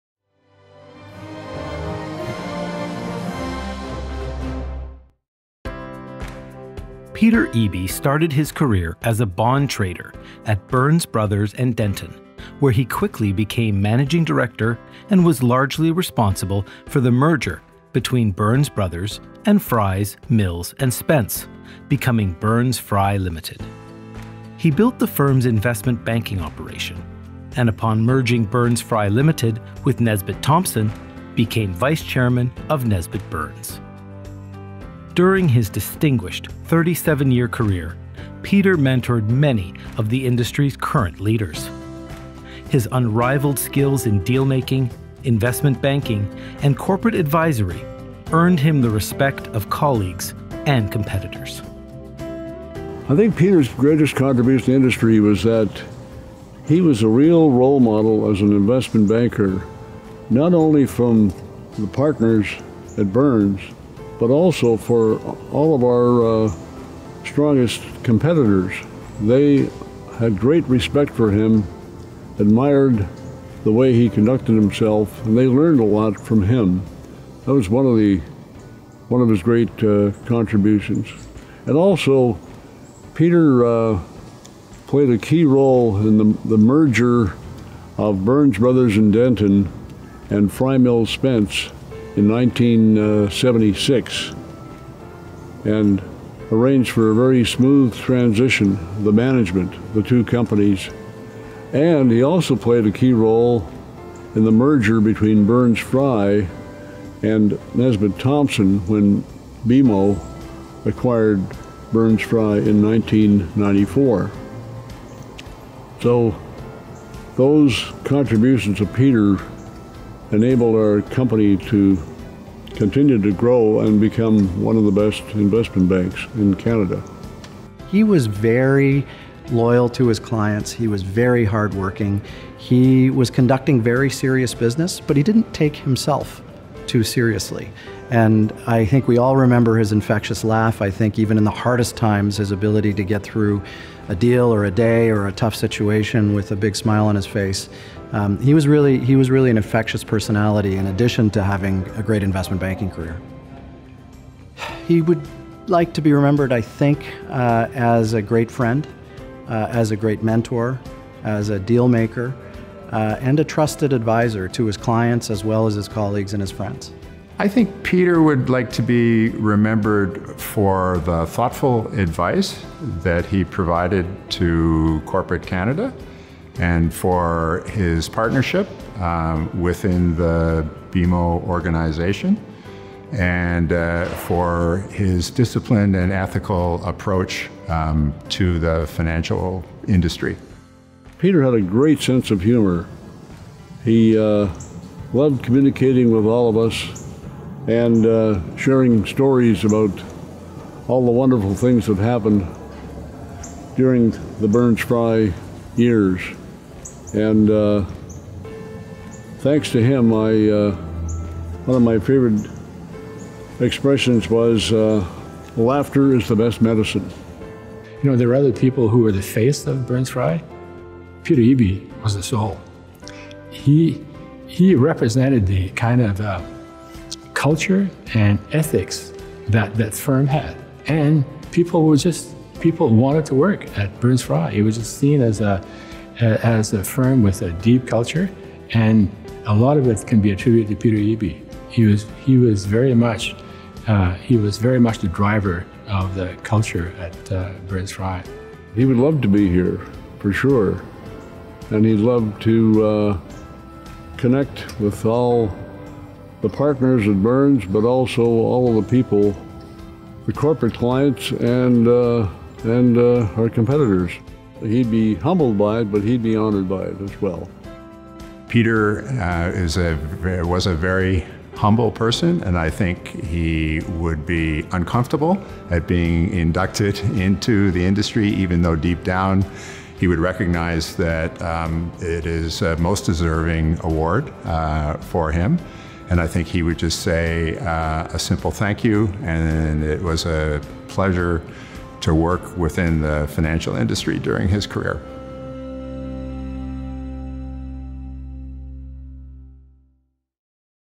Peter Eby 2021 Investment Industry Hall of Fame Inductee
After graduating from the University of California Berkeley, Peter took a job as a bond trader at Burns Bros. & Denton Ltd. He was quickly promoted up the ranks eventually becoming Managing Director. He helped to keep together the initially shaky merger between Burns Bros. and Fry, Mills & Spence – becoming Burns Fry Limited – and built the firm’s investment banking operation. Upon merging Burns Fry Limited with Nesbitt Thomson Inc., he became Vice- Chairman before semi-retiring in 1998. During his distinguished 37-year career, Peter mentored many of the current industry leaders. He always conducted himself with integrity, intelligence and graciousness. His unrivalled skills in deal-making, investment banking and corporate advisory earned the respect of colleagues and competitors.

Peter continued to be active as a corporate advisor, serving as a director of George Weston Ltd., Leon’s Furniture Ltd., and TD Asset Management USA Funds, Inc. He was past chair of the Olympic Trust of Canada and the Upper Canada College Foundation; past governor of the Toronto Stock Exchange and Upper Canada College; and the director of the AARC (Arthritis) Foundation and the Ontario Society of Crippled Children. His accomplishments, his service to his communities and his reputation were at the highest possible level.

Born in Toronto, Peter attended Upper Canada College and later the University of Toronto where he excelled at athletics, particularly football and hockey. He went on to earn his MBA from the University of California, Berkeley.